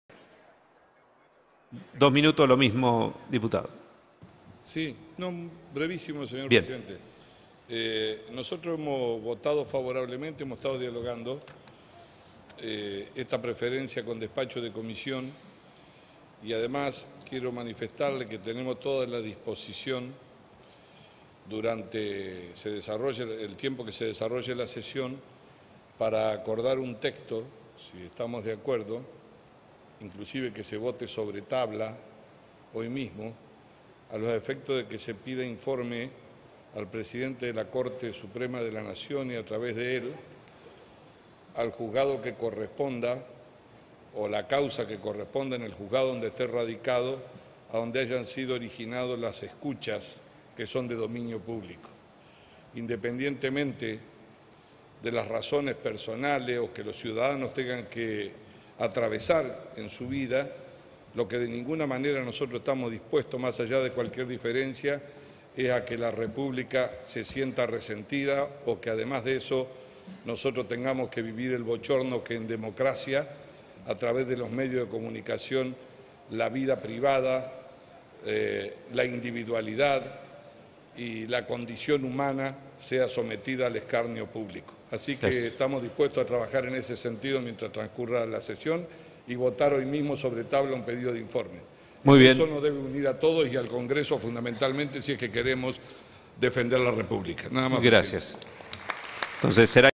Mario Negri - Sesión 21-03-2018 - Escuchas CFK Parrilli
El presidente del interbloque Cambiemos se refirió al pedido de informes de filtraciones recientemente difundidas en medios de comunicación entre la ex presidenta Cristina Fernández de Kirchner y Oscar Parrilli